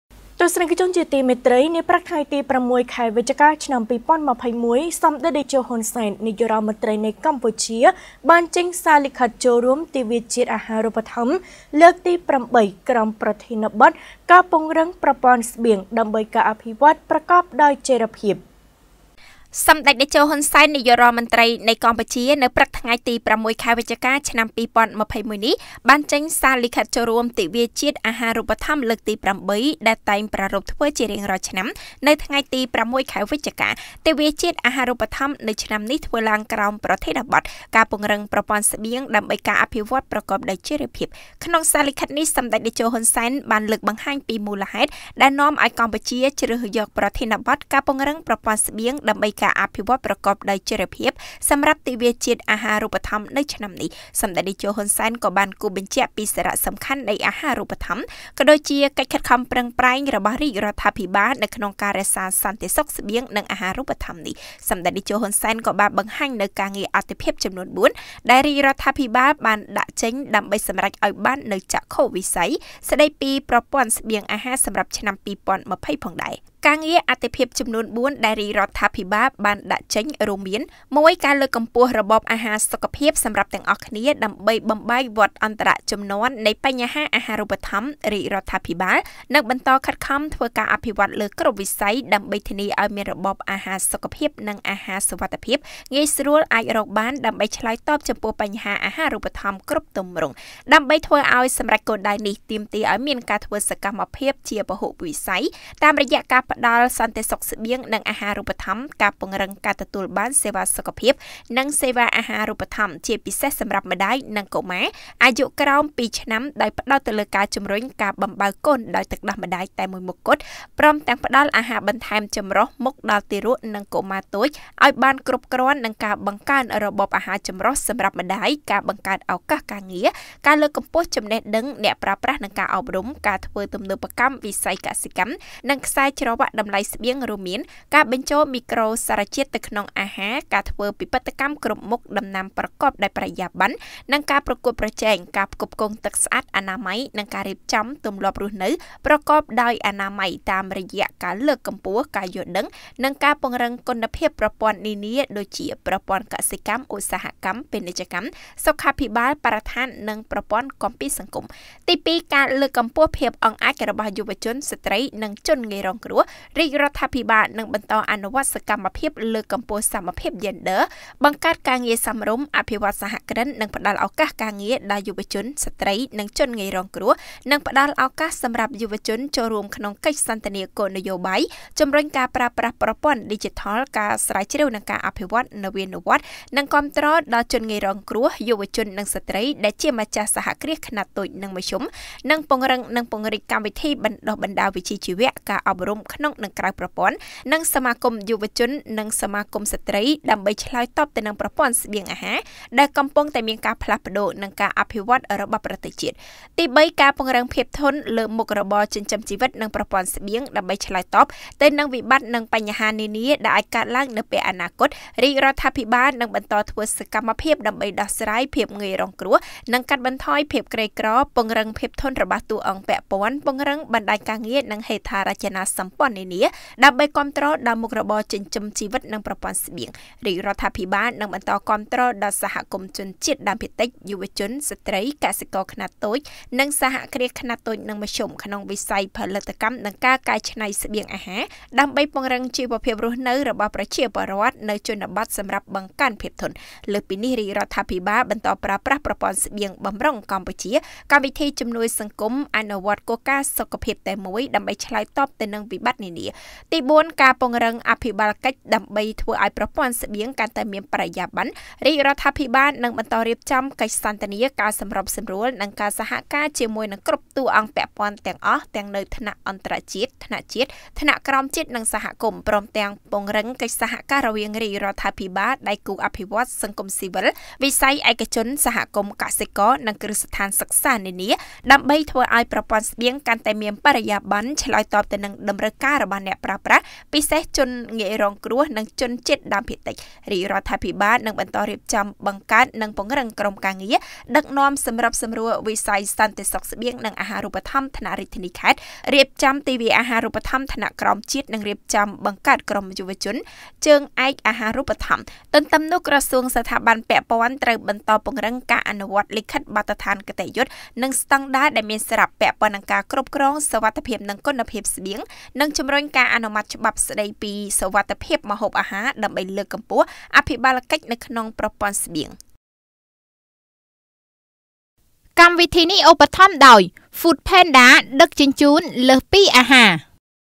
សម្តេចតេជោ ហ៊ុន សែន ចេញសារលិខិតចូលរួមទិវាជាតិអាហារូបត្ថម្ភ លើកទី៨ ក្រោមប្រធានបទ «ការពង្រឹង...
សម្តេចតេជោ ហ៊ុន សែន ចេញសារលិខិតចូលរួមទិវាជាតិអាហារូបត្ថម្ភ លើកទី៨ ក្រោមប្រធានបទ «ការពង្រឹងប្រព័ន្ធស្បៀង ដើម្បីការអភិវឌ្ឍប្រកបដោយចីរភាព»
* រាយការណ៍ព័ត៌មានដោយកញ្ញា តាំង វិគី និងអ្នកនាង  យ៉ង់ សក្គុណា , ទូរទស្សន៍ព័ត៌មាន Fresh News